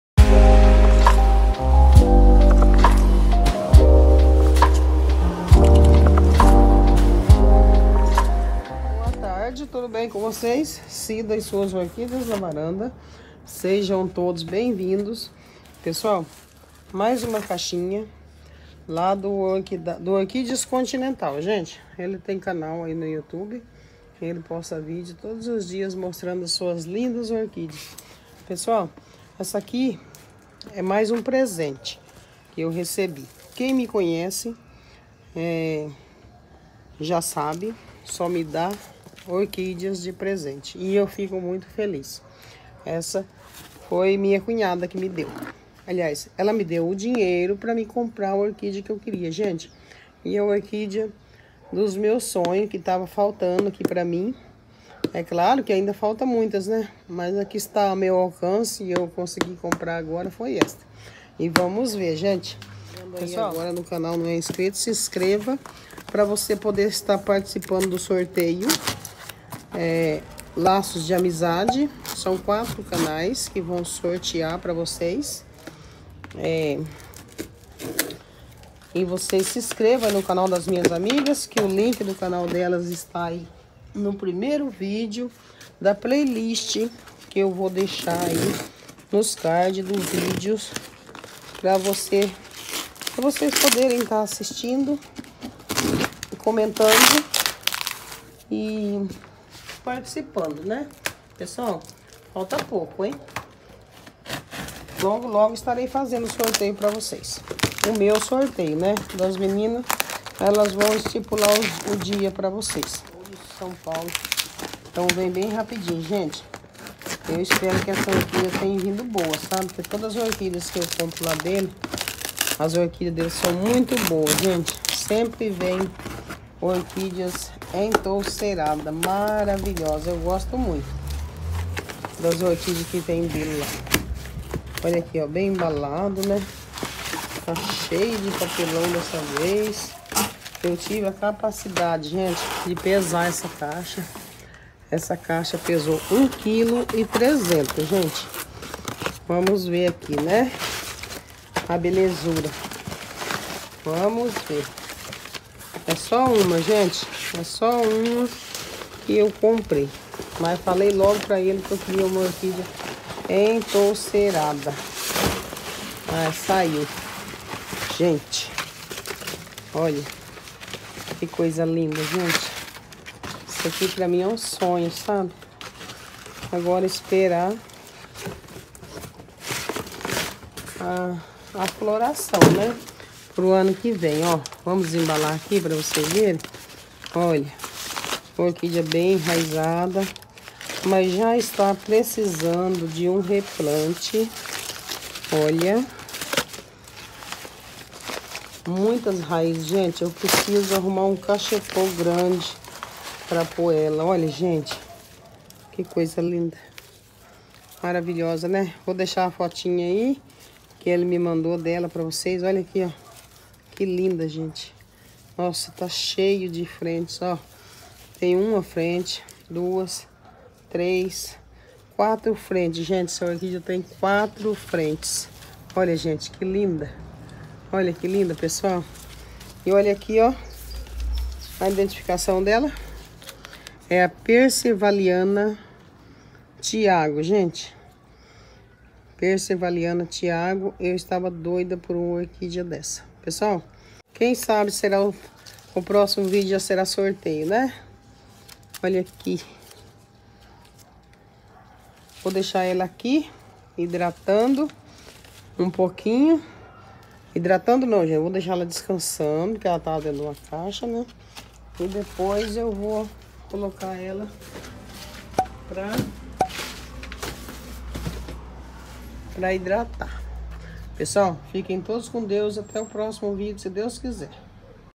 Vamos Abrir Mais Uma Caixa De Orquideas ?
Orquideas recebidos do Orquideas continental!

Vamos abrir a caixa?

Vamos ver como veio a Orquídea percevaliana?

Olá Pessoal tudo bem com vocês no vídeo de hoje vamos abrir mais uma caixinha essa caixinha gente fica muito feliz quando eu abri uma caixinha dessa caixinha de orquídeas pessoal se aqui é o aniversário passou já tem um mês mais e ainda é um presente de aniversário foi minha cunhada que me deu ela me deu dinheiro eu comprei lá no orquídeas Continental o pessoal pediu moro aqui dia bem estou hospedada vamos ver se veio boa

né é só lá no aqui diz que eu gosto muito de compra lá porque as orquídeas dele são maravilhosas gente sempre vem orquídeas então será anda e orquídeas￼ Muito boas espero que vocês gostem do vídeo compartilhe com seus amigos obrigado a todos￼